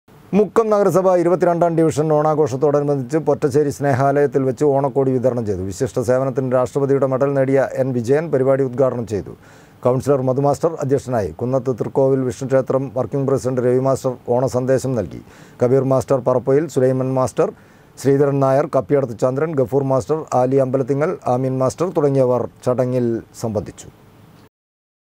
ഓണാഘോഷത്തോടനുബന്ധിച്ചു പൊറ്റശ്ശേരി സ്നേഹാലയത്തിൽ വെച്ച് ഓണക്കോടി വിതരണം ചെയ്തു
മുക്കം നഗരസഭാ ഇരുപത്തിരണ്ടാം ഡിവിഷൻ ഓണാഘോഷത്തോടനുബന്ധിച്ചു പൊറ്റശ്ശേരി സ്നേഹാലയത്തിൽ വെച്ച് ഓണക്കോടി വിതരണം ചെയ്തു.